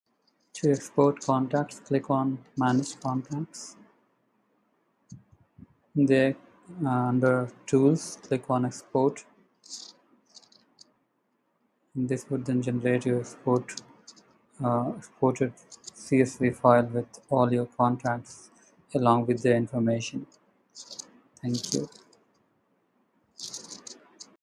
How to export contacts in Dryfta event CRM
Please note that Dryfta is continuously updated, so the dashboard you see in your account may not exactly match the one depicted in this video.